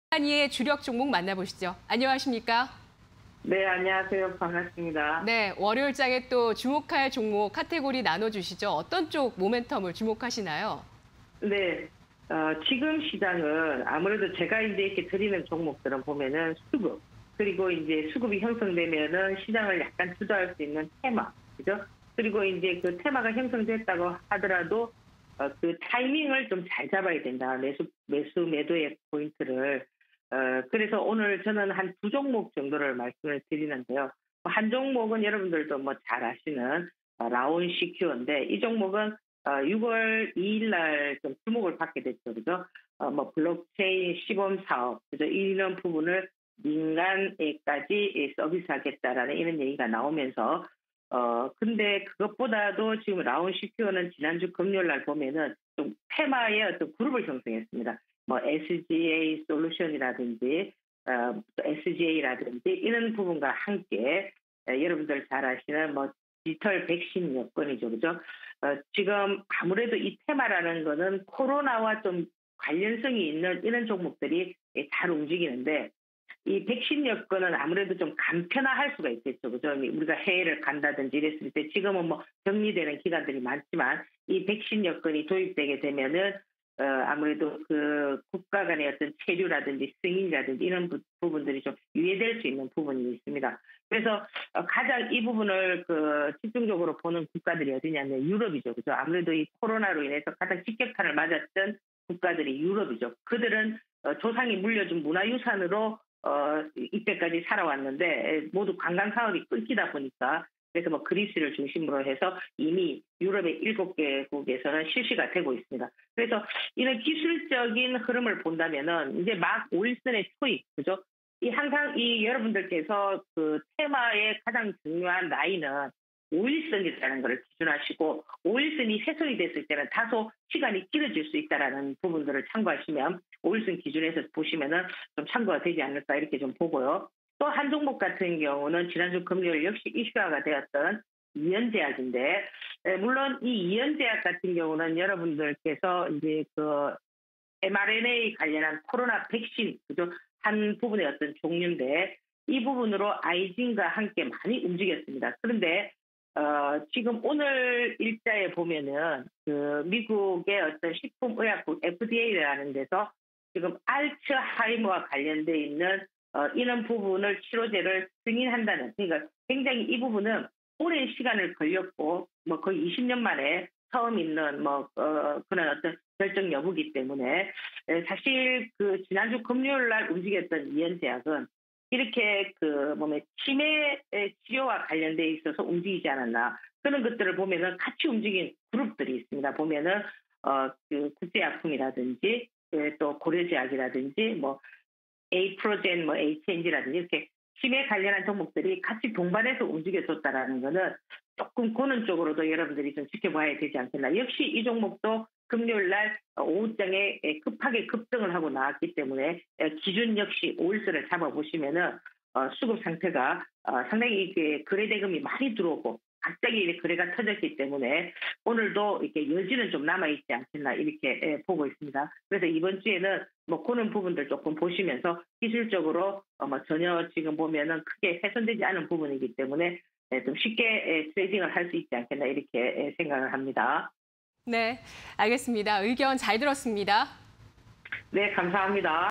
라온시큐어, 이연제약, SGA솔루션즈_이난희의 주력 종목 (20210607)
1. 라온시큐어(042510)
2. SGA솔루션즈(184230)
3. 이연제약(102460)

라온시큐어(042510)
- 소프트웨어 개발 판매 기업
- IT통합보안·인증 기업
- 디지털 백신여권 관련주

이연제약(102460)
- 원료의약품 제조 기업
- mRNA 백신 관련주
- 치매 관련주